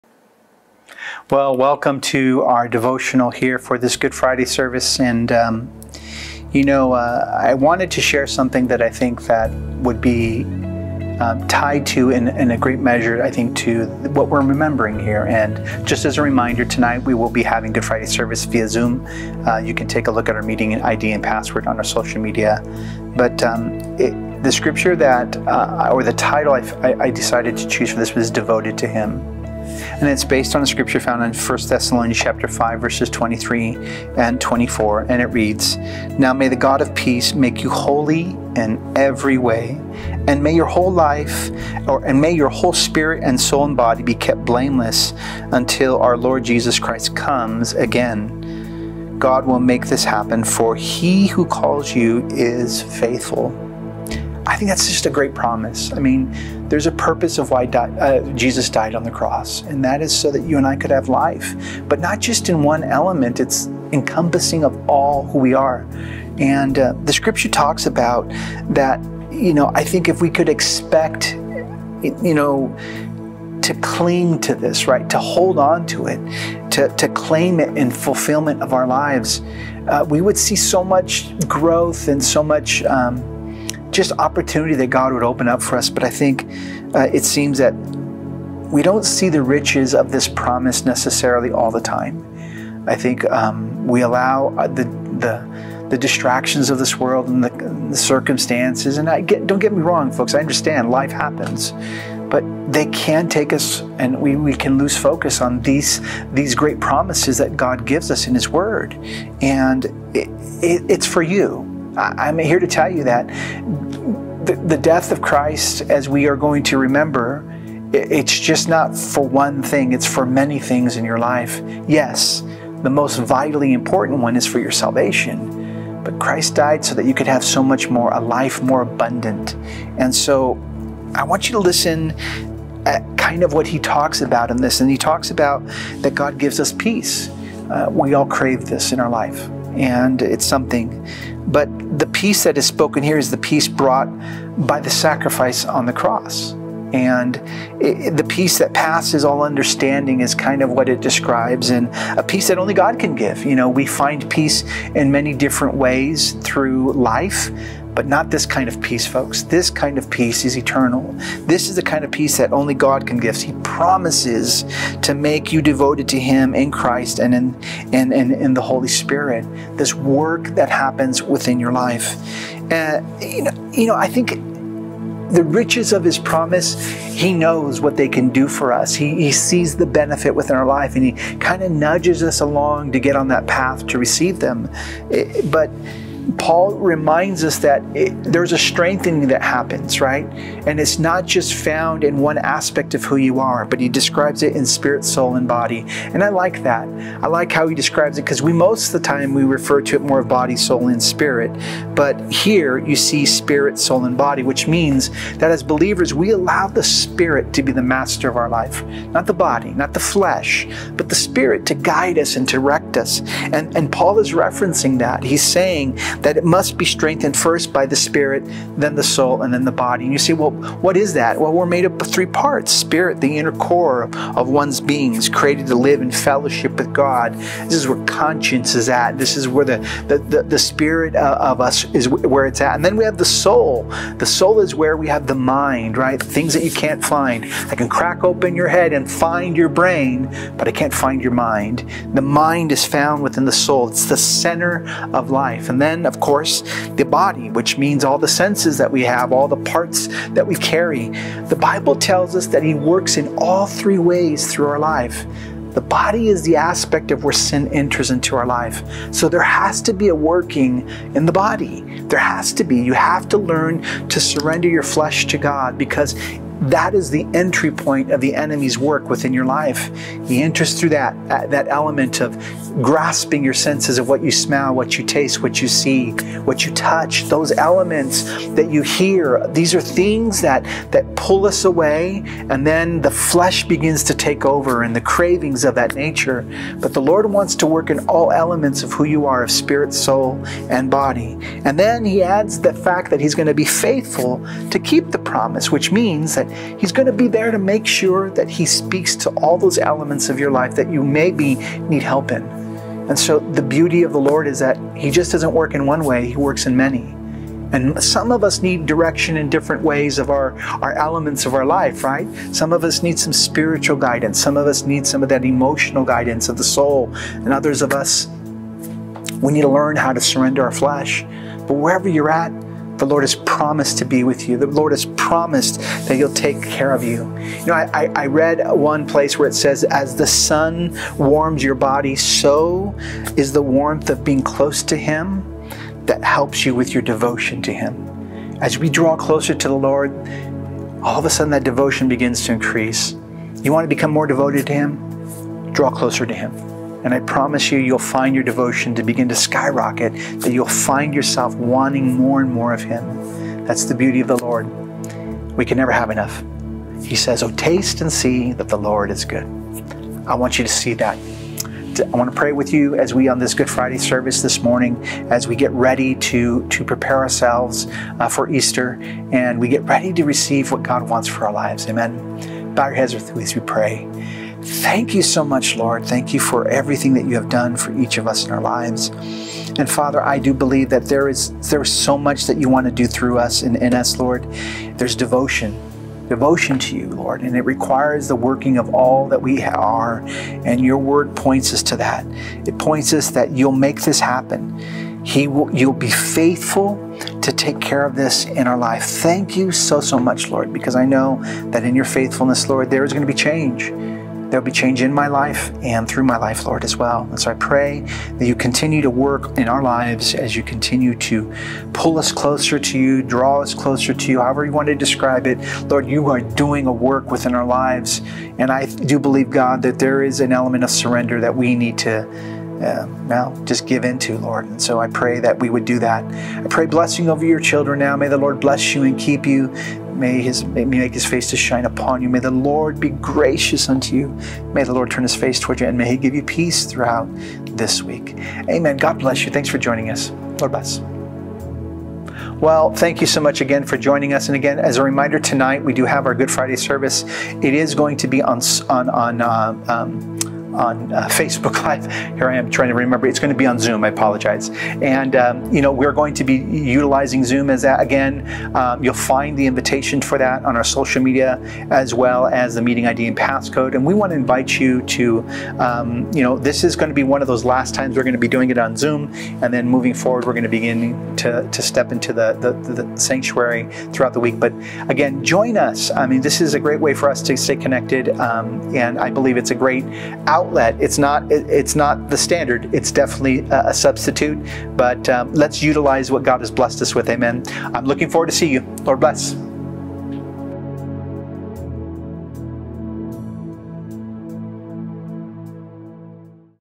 Daily Devotional-Devoted to Him | Pastor Craig Sanchez | Faith Tabernacle | 04/07/2023
Join us for our devotional on this Good Friday as Pastor encourages us that we should be devoted because the peace He has brought us.